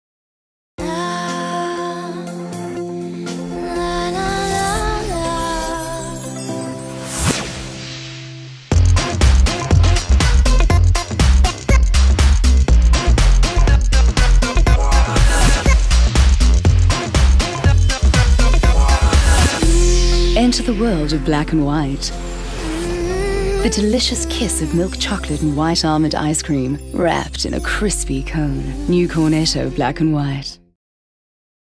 Cornetto Black and White: Commercial Voice Over
Jo Galloway voices the commercial for New Cornetto Black and White.
This clip is for Show Reel  and Demo purposes only and not for commercial advertisement.